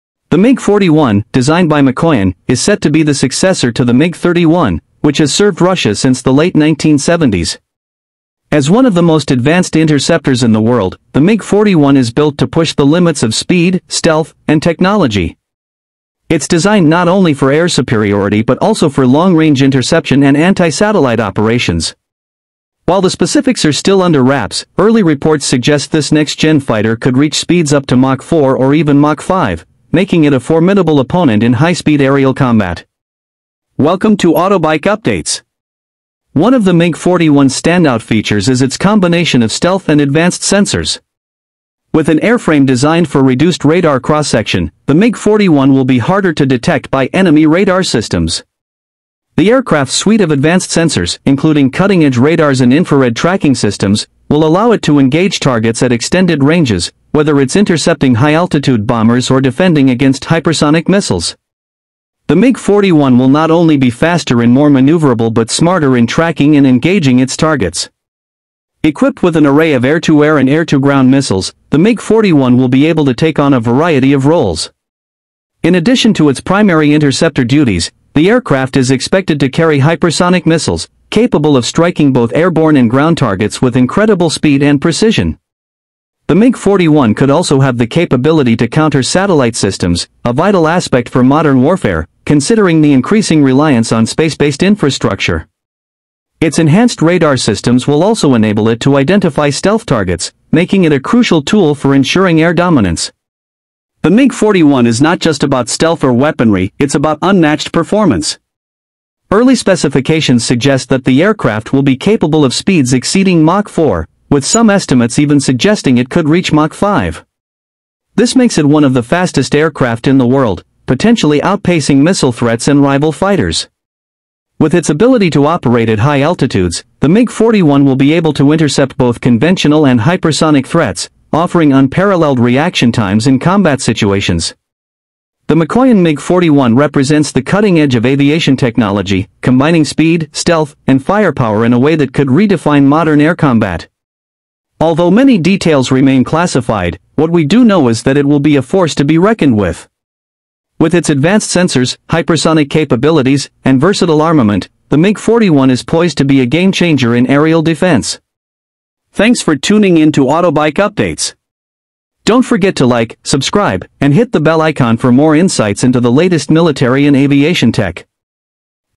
Original video original voice 🔥 🚀 Mikoyan MiG-41 – Russia’s Next-Gen Hypersonic Stealth Fighter! 🛩️
🚀 Mikoyan MiG-41 – Russia’s Next-Gen Hypersonic Stealth Interceptor Fighter! 🛩️

The Mikoyan MiG-41, also known as the PAK DP, represents the future of Russian air defense. Designed to replace the legendary MiG-31, this 6th-generation stealth interceptor is expected to achieve speeds beyond Mach 4, operate at near-space altitudes, and integrate advanced hypersonic missile systems. In this video, we explore its design, capabilities, and how it’s shaping the next era of air superiority.